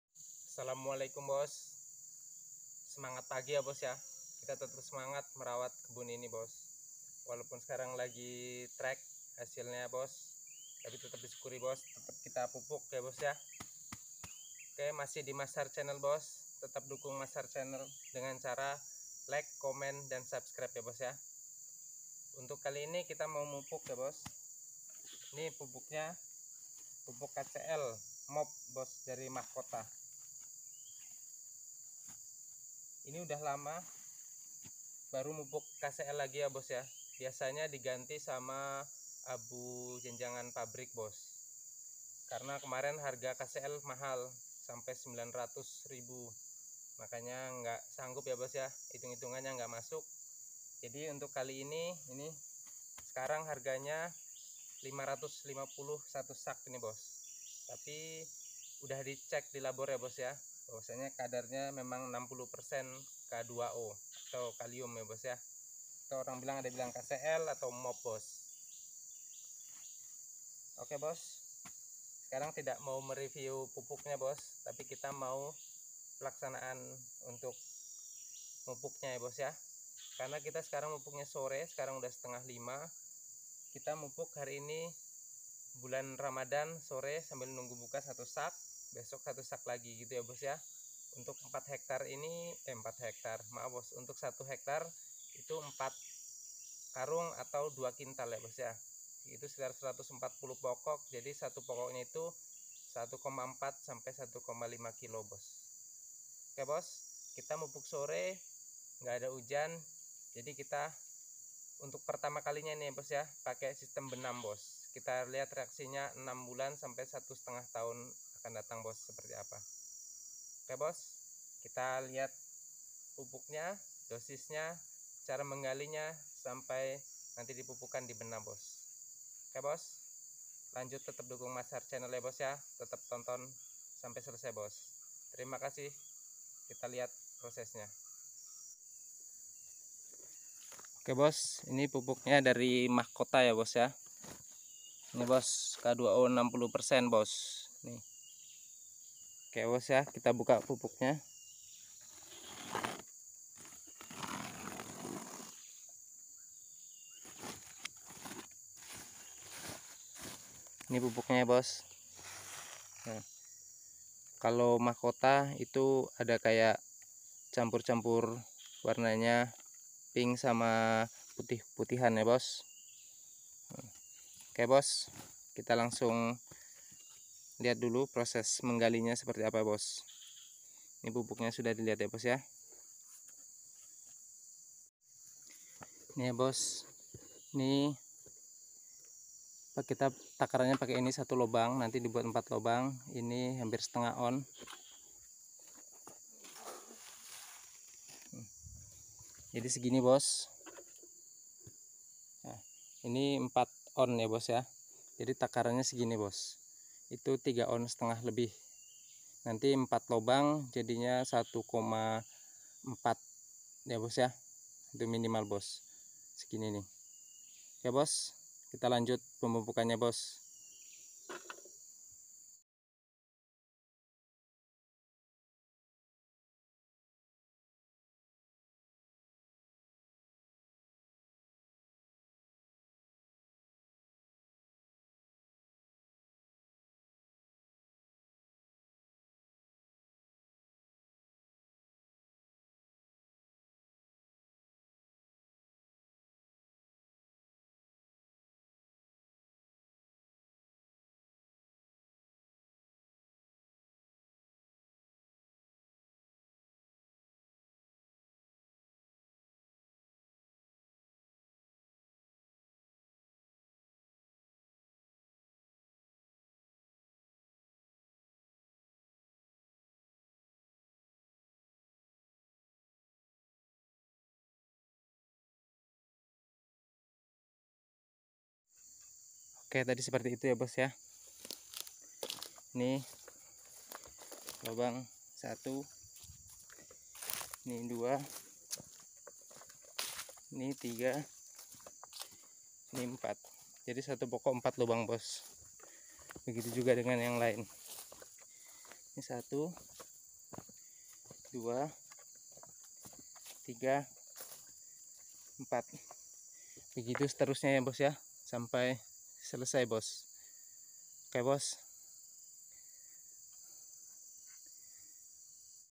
Pemupukan Kelapa Sawit Urutan ke Empat dengan Cara di Benam
Pemupukan Kelapa Sawit Urutan ke Empat Pupuk KCL/MOP dengan Cara di Benam